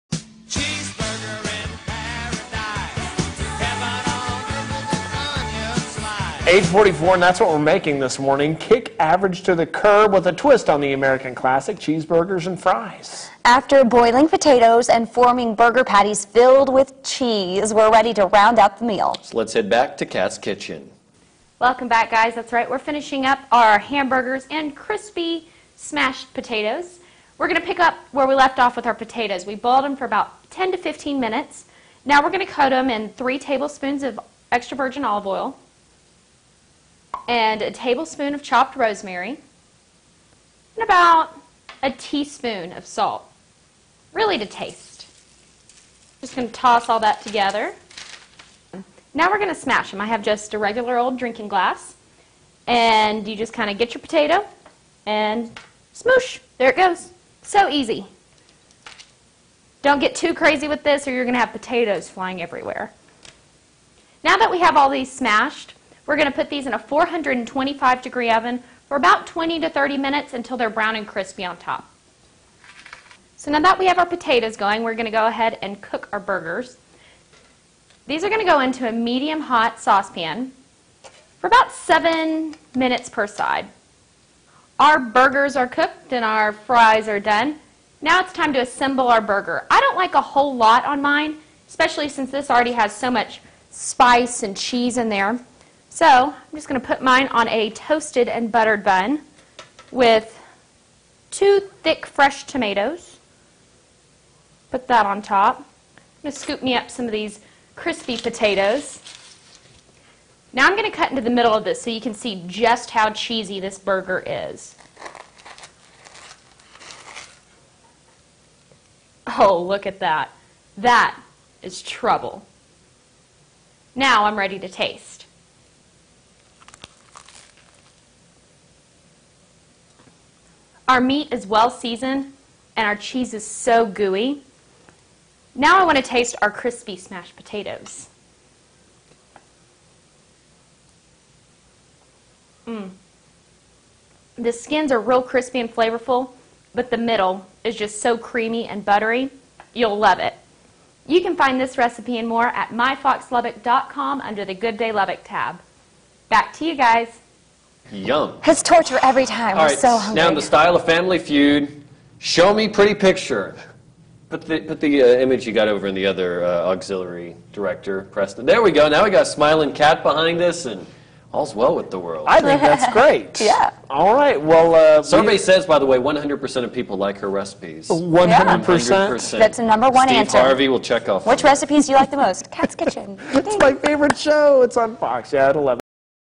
Kat's Kitchen- Cheeseburgers and Smashed Potatoes, pt. 2
A playful twist on cheeseburgers and french fries.